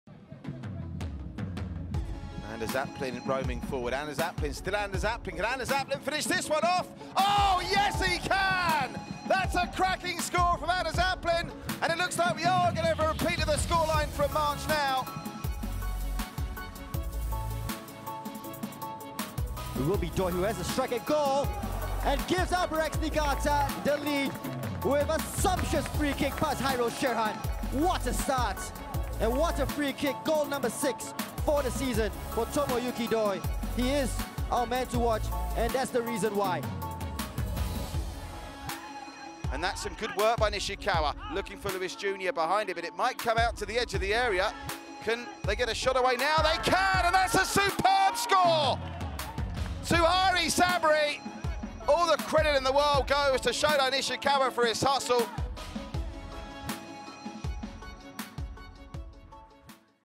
Top 3 SPL Goals in 60 Seconds (2020 Season - Round 10)
🔥 Top 3 SPL Goals in 60 Seconds (Round 10) ⏱

🙋‍♂️ Who here loves strikes from distance? We've got you covered with these beauties from Anders Aplin (Hougang United Football Club), Tomoyuki Doi (Albirex Niigata FC Singapore) & Suhairi Sabri (Tanjong Pagar United F.C.). 🚀